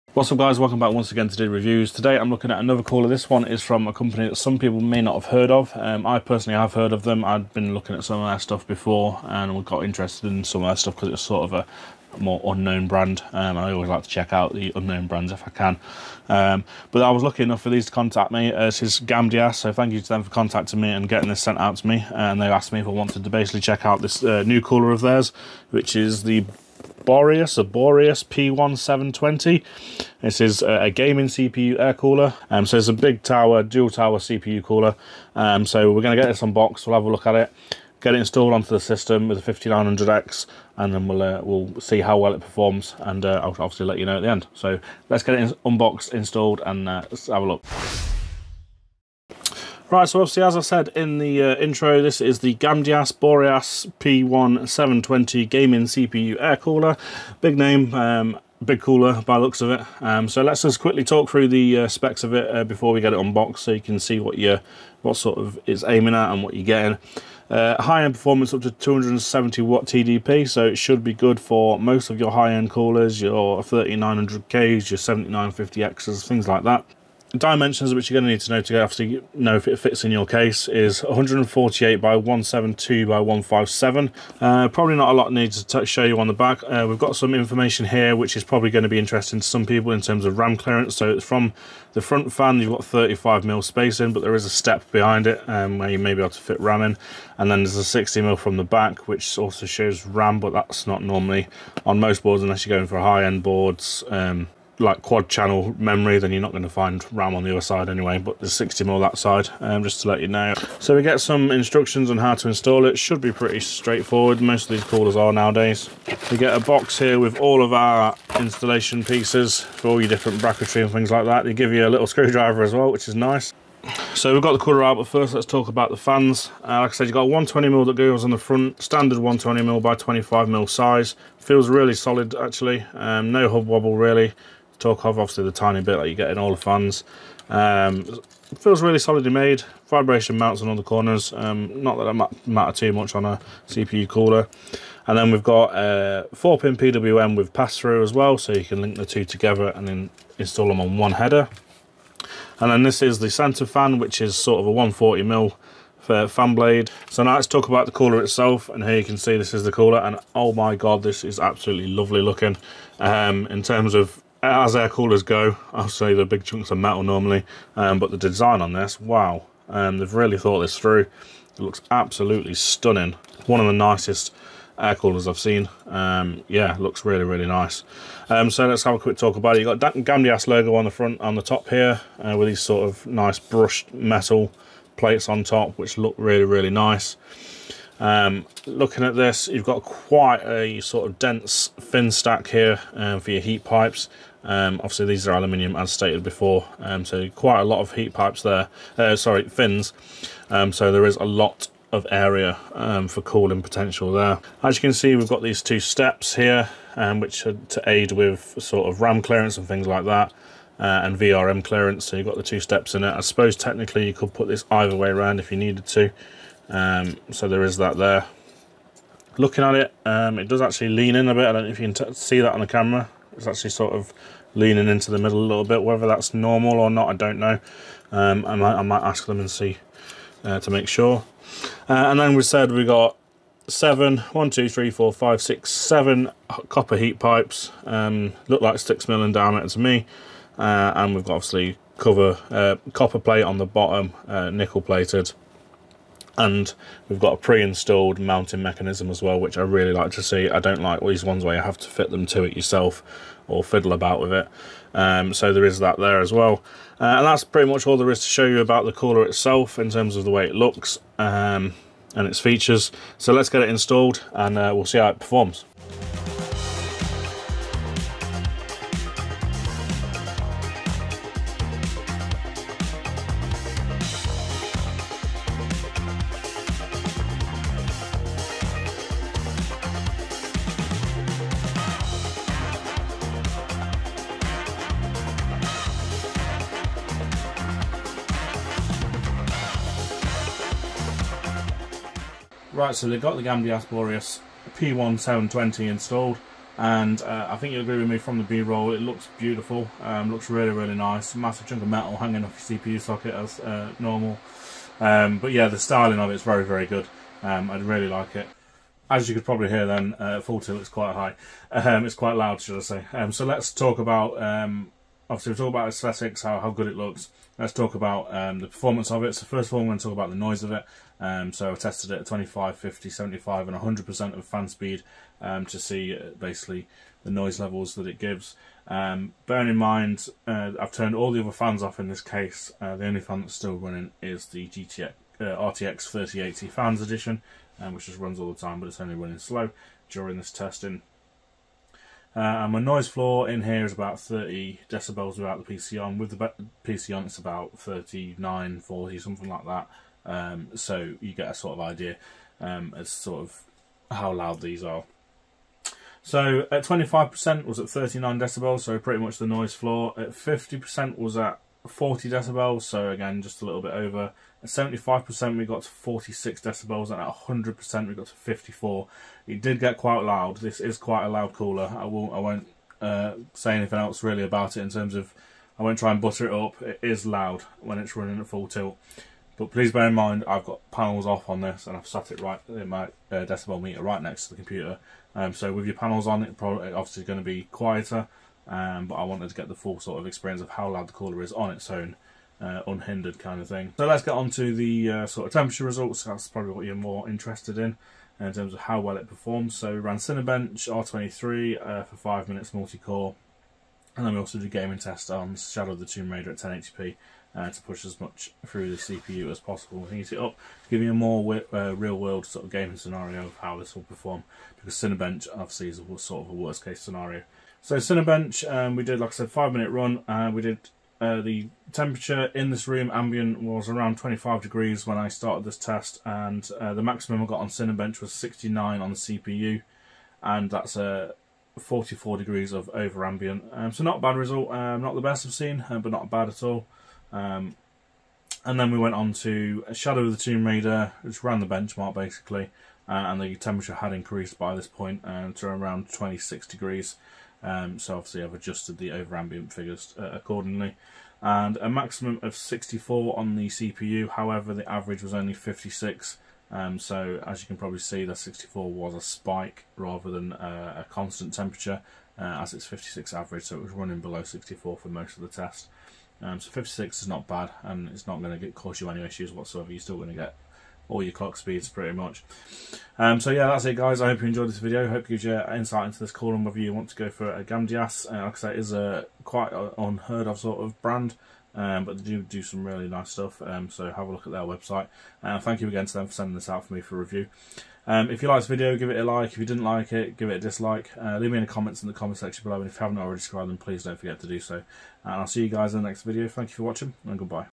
Best CPU Cooler? Gamdias Boreas P1 720 Gaming CPU Cooler Review
Is this new cooler from gamdias the best cpu air cooler?

named as a gaming cpu cooler, it is absolutely massive and has 2 fans, 120 and 140mm fans to cool your cpu.

can this perform as good as the likes of noctua, thermalright and be quiet?

lets see what the temperatures are like and whether it is worth buying.

Thank you to @GamdiasTechnology for sending this over for review

00:00:00 Intro
00:00:50 Specs
00:01:54 Unboxing
00:04:25 How good does it look
00:05:34 Sound levels
00:07:06 Cinebench results
00:07:19 Gaming results